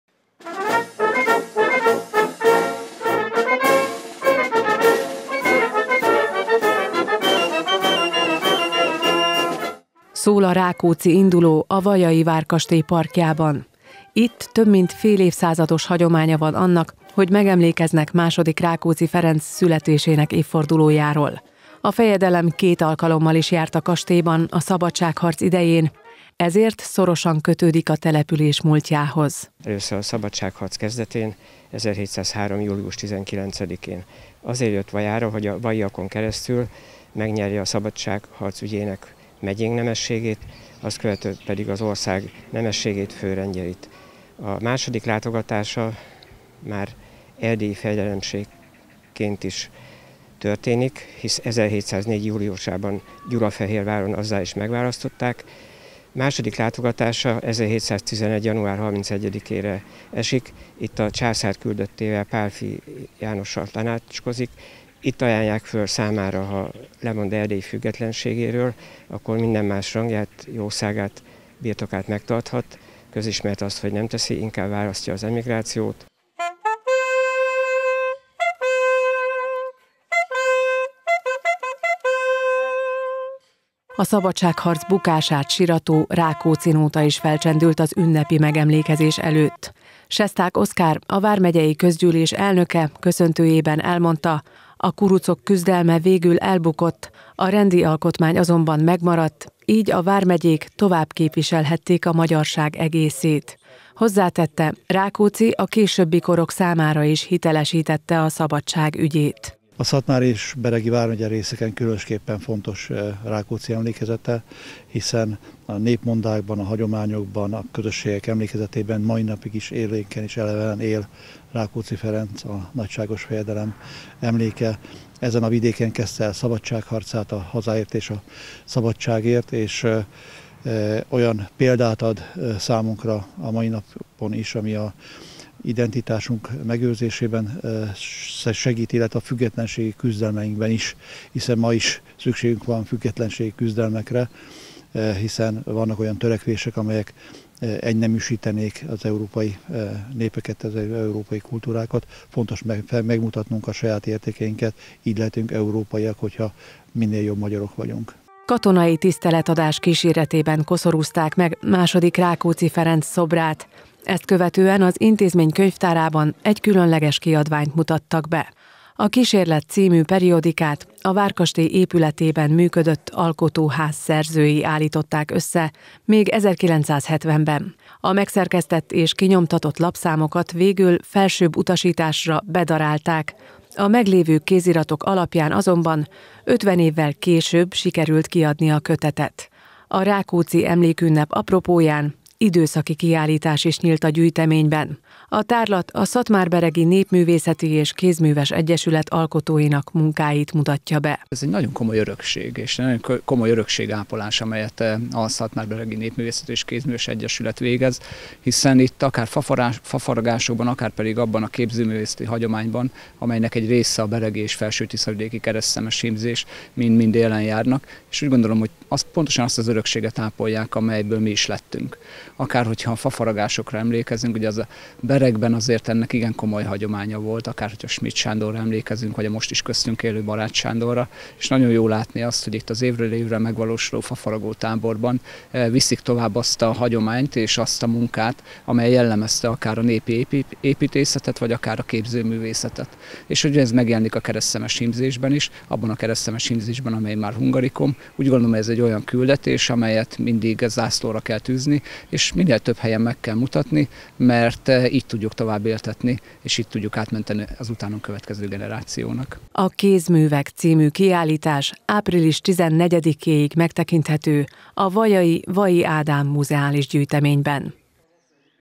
Rákóczi-emlékünnepség
II. Rákóczi Ferencre emlékeztek a vajai várkastélyban. A fejedelem születésének 348. évfordulója alkalmából rendezett ünnepségen elhangzott: Rákóczi a későbbi korok számára is hitelesítette a szabadság ügyét. A rendezvényen egy majdnem feledésbe merült kiadványt is bemutattak. A várkastélyban tárlat is nyílt, amely a Szatmár-Beregi Népművészeti és Kézműves Egyesület alkotóinak munkáit mutatja be.